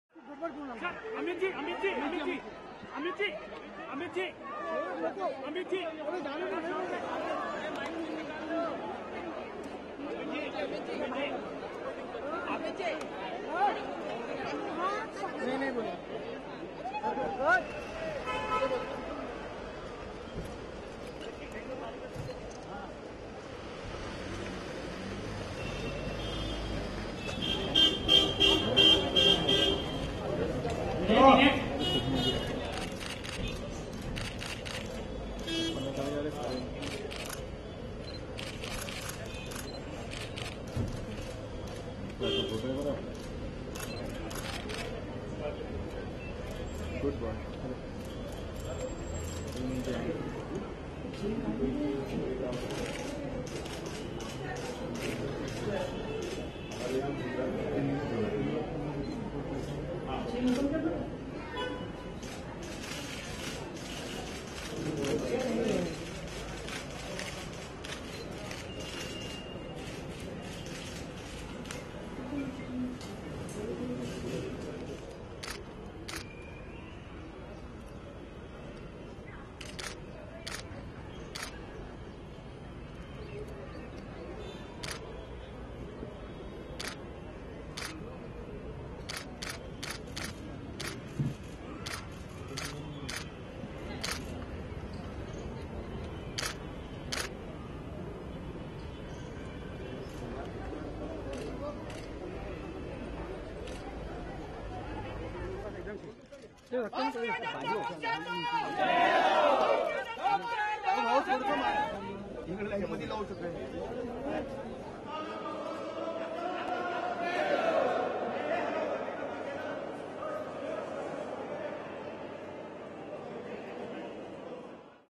BJP President visits India's Hindu organisation headquarters to plan ahead of assembly elections
The new president of India's ruling Bharatiya Janata Party (BJP) arrives in western Maharashtra at the headquarters of Hindu organisation Rashtriya Swayamsevak Sangh (RSS), to plan out the party's strategy ahead of the upcoming assembly elections.